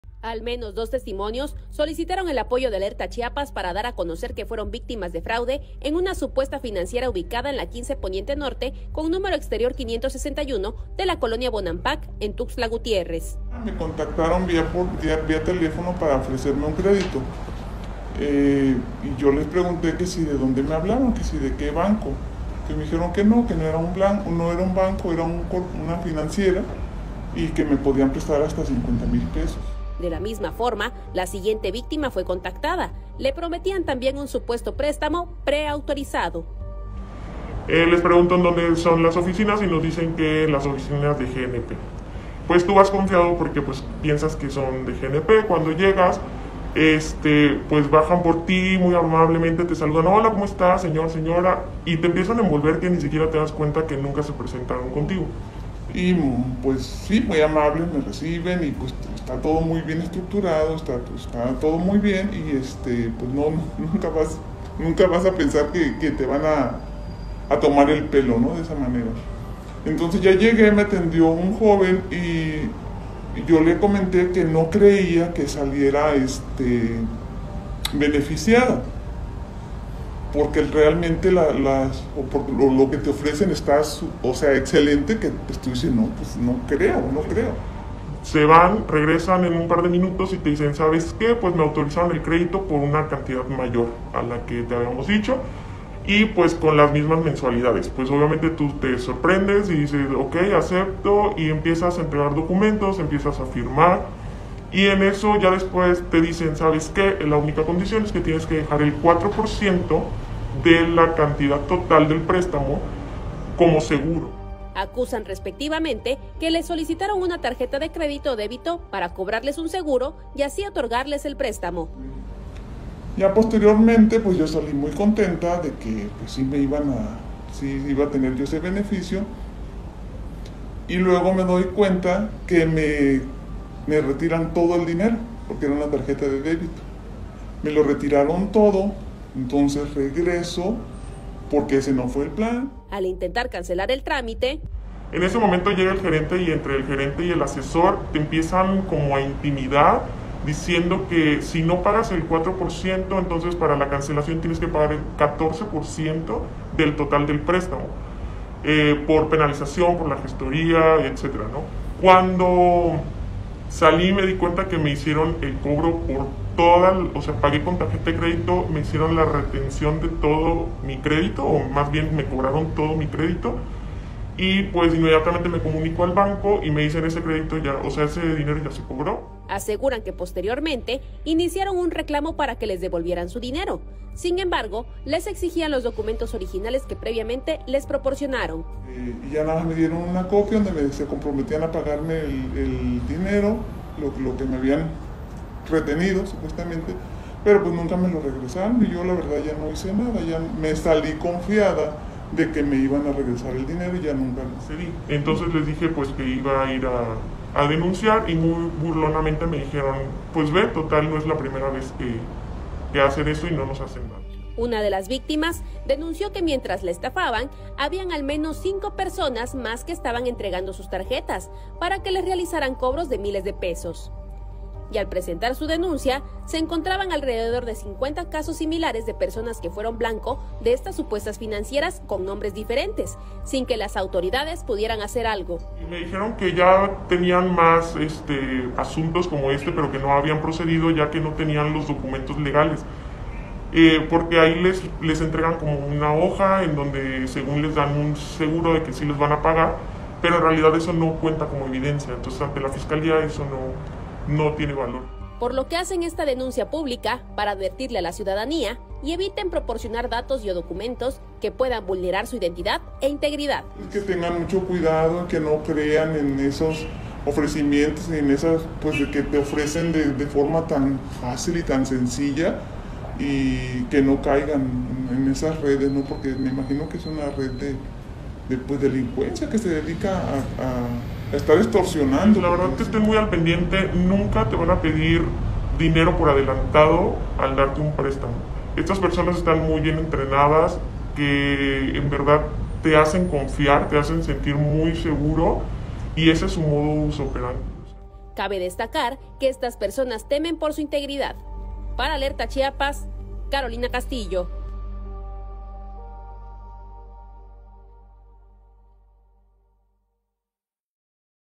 Fueron víctimas de una financiera fraudulenta en Tuxtla.
Les ofrecieron un crédito por una cantidad mayor a la solicitada pero les pidieron pagar el 4% del total del préstamo así como tarjetas de crédito o débito para cobrar un seguro, pero se las terminaron vaciando.